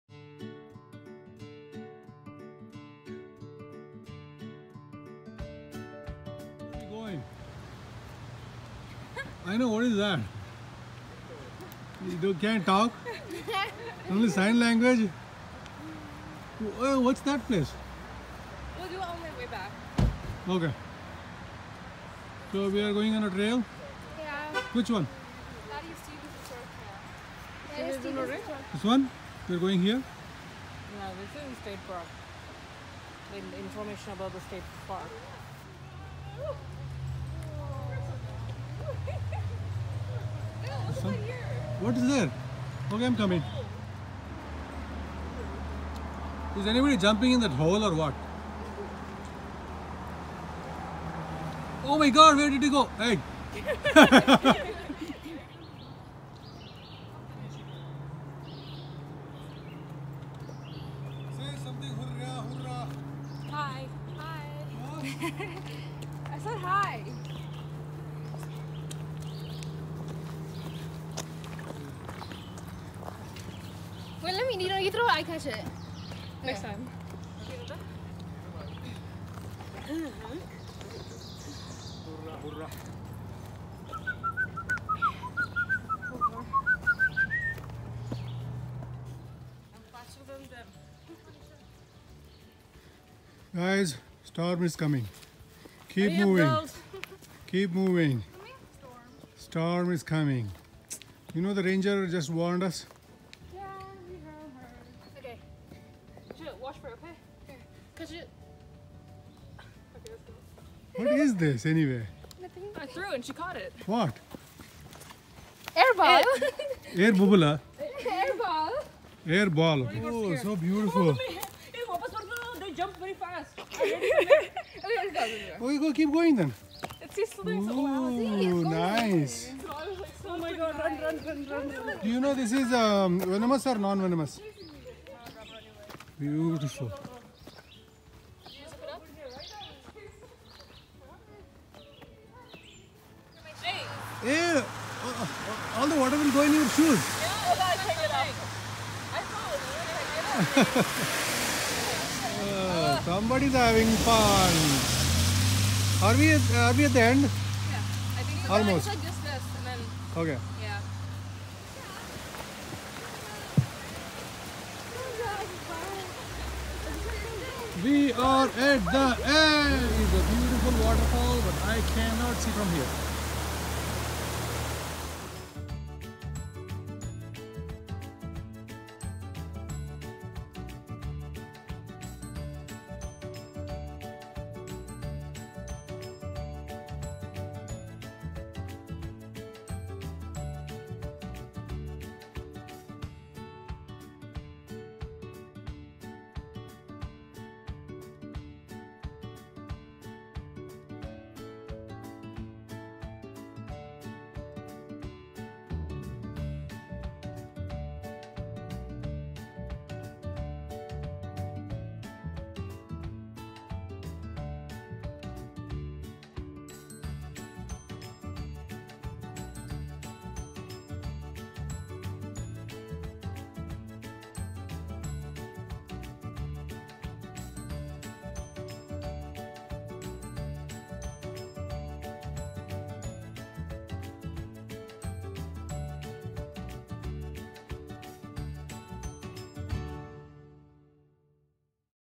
Charcoal Hearth Trail, Caledonia State Park, Pennsylvania
Family hiking on Charcoal Hearth Trail located in Caledonia State Park, Pennsylvania